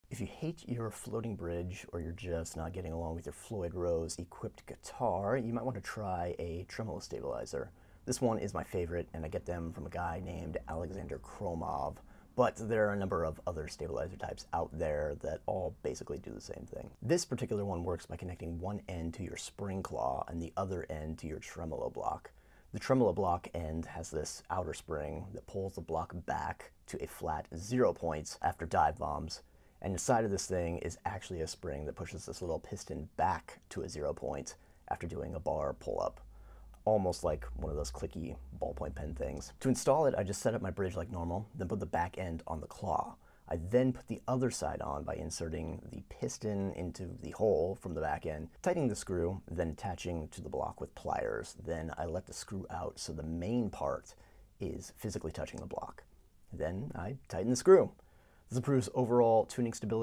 Hate your Floyd Rose guitar? Try a tremolo stabilizer.
If you hate your floating bridge, or are just not getting along with your Floyd Rose equipped guitar, you might want to try a tremolo stabilizer. This one is my favorite, and I get them from a guy names Alexander Khromov, but there are a number of other stabilizer types out there that all basically do the same thing. This particular one works by connecting one end to your spring claw, and the other end to your tremolo block. The tremolo block end has this outer spring that pulls the block back to a flat 0 point after dive bombs, and inside of this thing is actually a spring that pushes this little piston back to a 0 point after doing a bar pull-up (almost like one of the clicky ballpoint pens). To install it, I just setup my bridge like normal, then put the back end on the claw. I then put the other side on by inserting the piston into the hole, tightening the screw, attaching to the block with pliers, then letting the screw out so the main part is physically touching the block. Then I tighten the screw. This improves overall tuning stability, and makes for much less annoying operation.